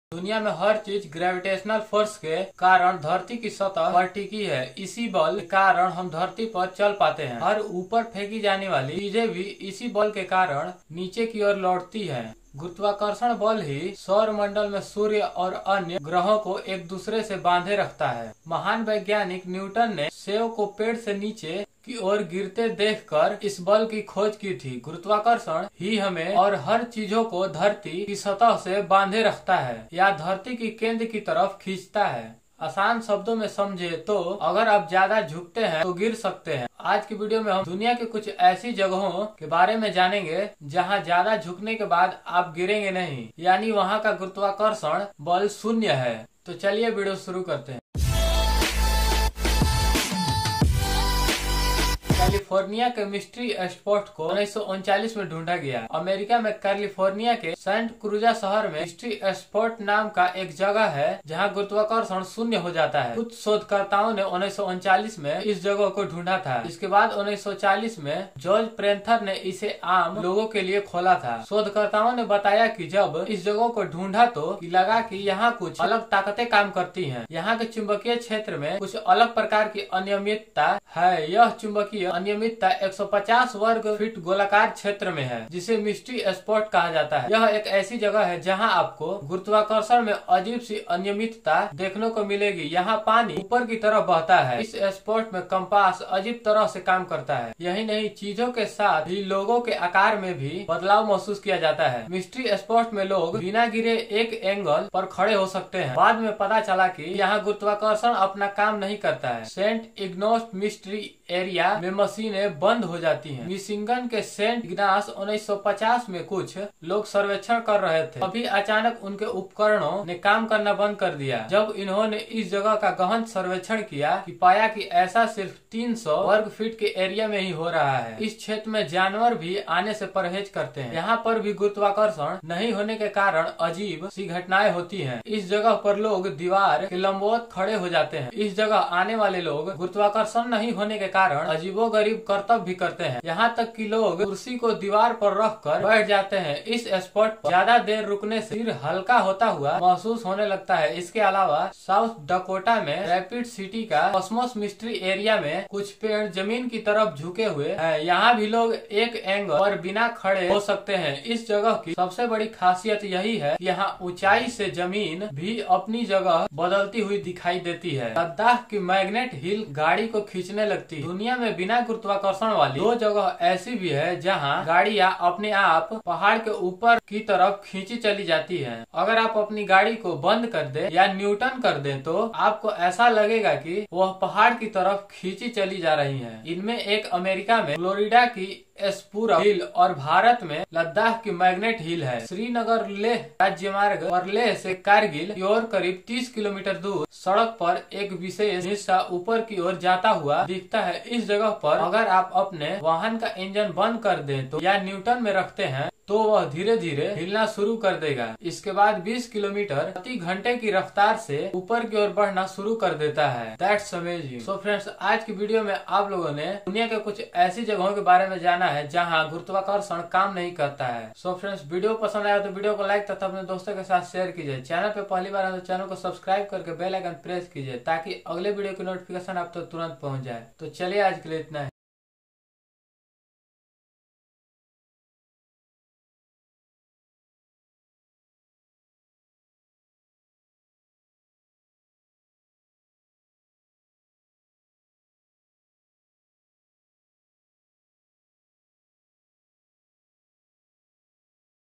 दुनिया के रहस्यमय जगह जहाँ Gravity काम नहीं करती | Top Place In World Without Gravity
Mera name Imran Ansari Hai. Me Aap Logon ko Duniya ke Kuch Aishe Place Ke Bare Me Bataunga Jha Gravity Kam Nhi karti Hai.